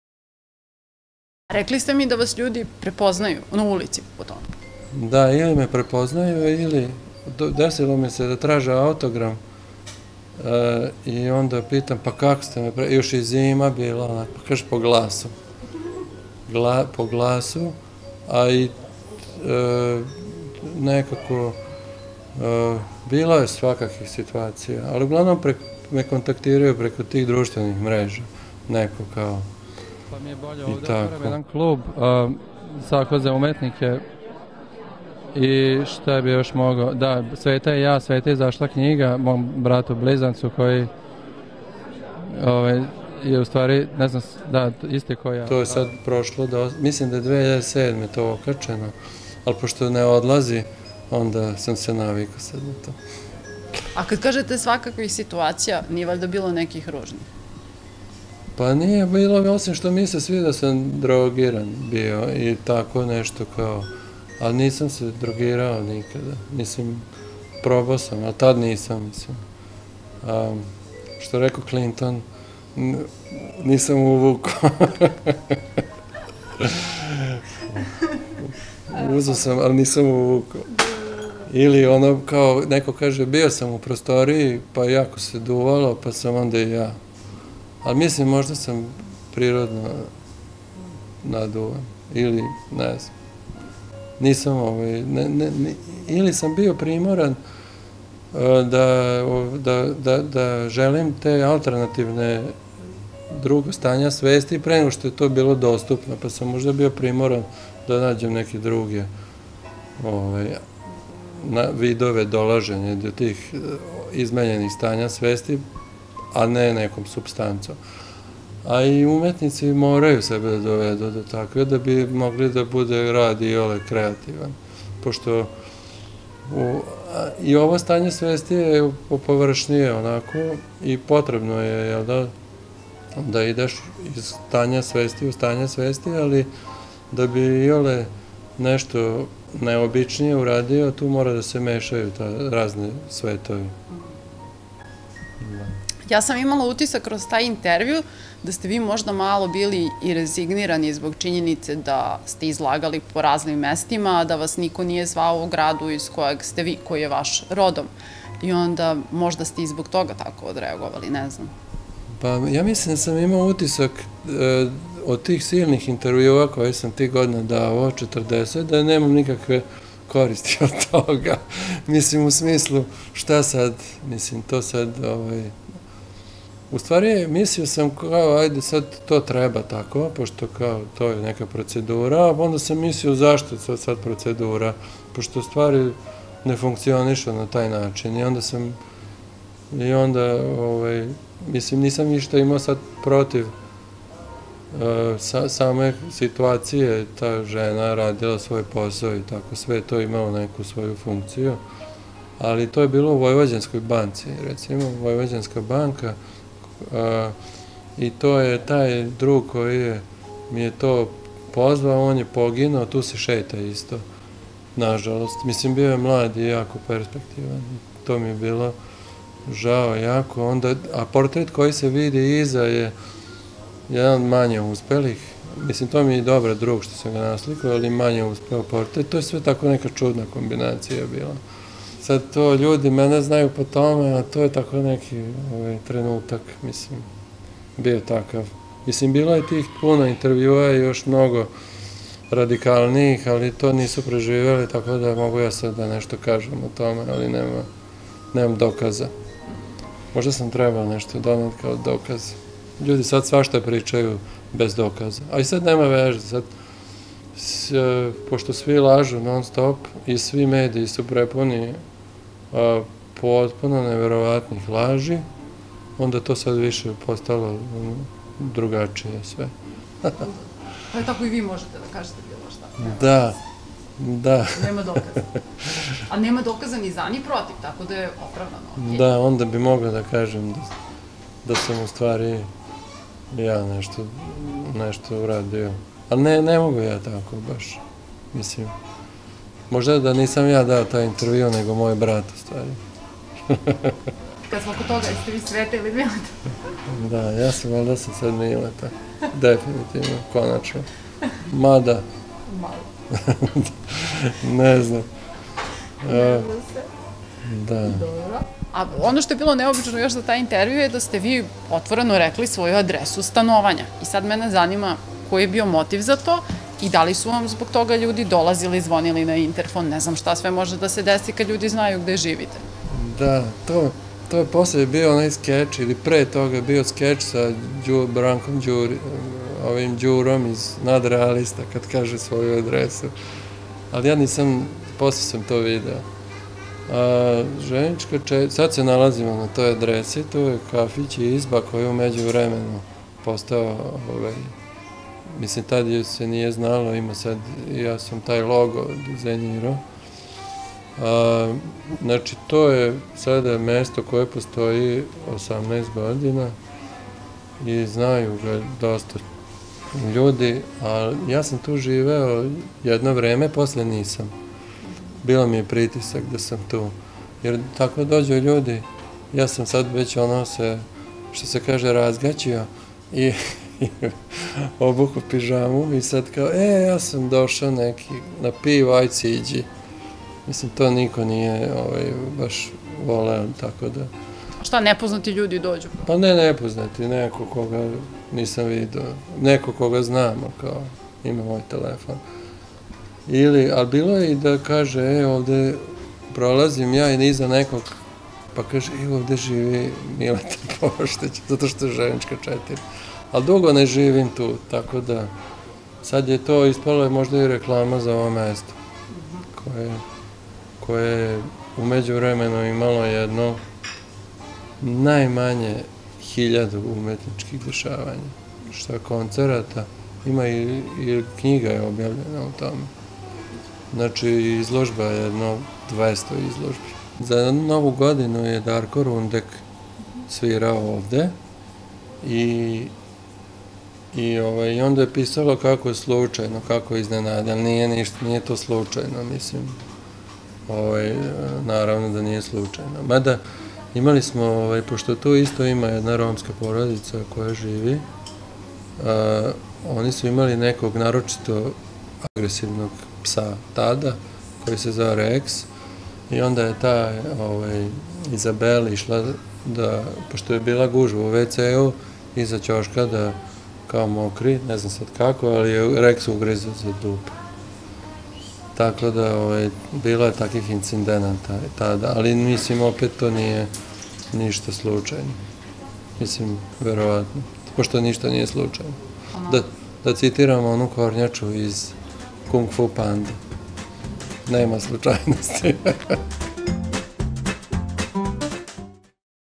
SVETA ili MILETA - 18 godina kasnije (DRUGI DEO) | Mondo TV
Mileta Poštić, umetnik i animator iz Novog Sada, postao je poznat najširoj publici svojim nesvakidašnjim intervjuom koji se pojavio na Jutjubu pre 10 godina i postao hit...